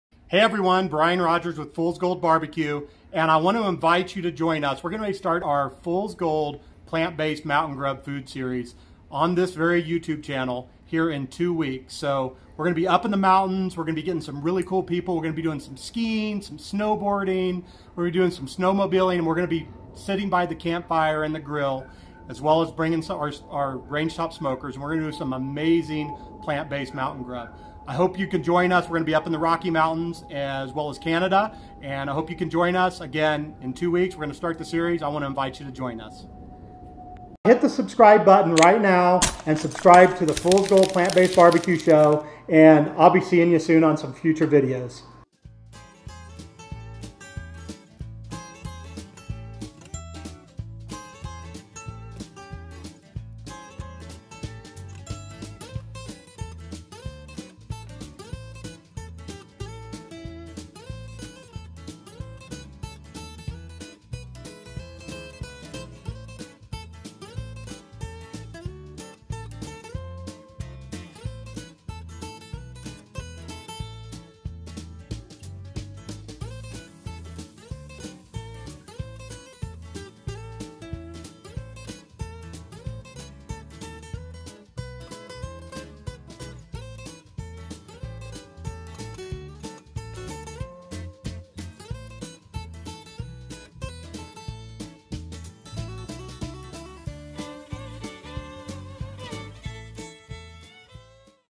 Mountain Grub Video Series | Coming soon | Fool's Gold Plant-based BBQ
Welcome back to America’s #1 Plant - Based BBQ channel. We have exciting news for you. Soon, you will join Brian on his brand new Mountain Grub video series in which he will share his experiences in the Rocky Mountains and Canada.

Want more plant-based recipes or want to know more about my story of how I became into a BBQ legend and the #1 plant-based BBQ expert in the world? Please find more on my website and  blog: